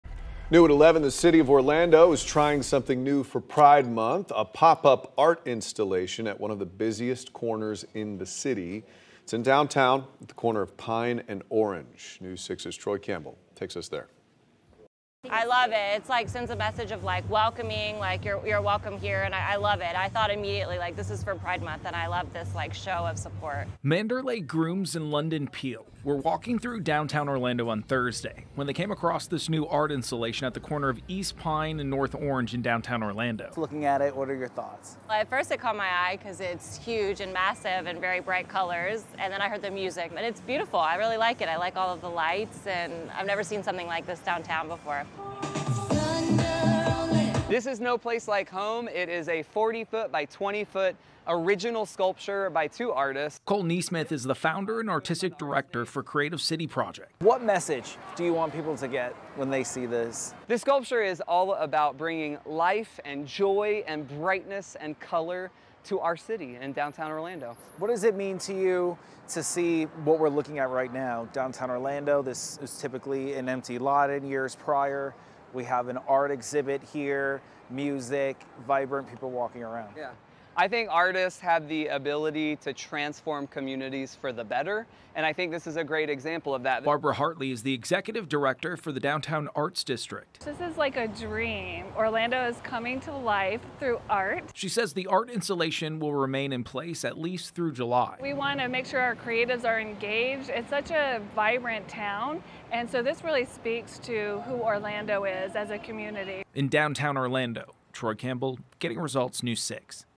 Pop-up art installation put up in downtown Orlando
A new art installation in downtown Orlando has been completed in time for Pride month.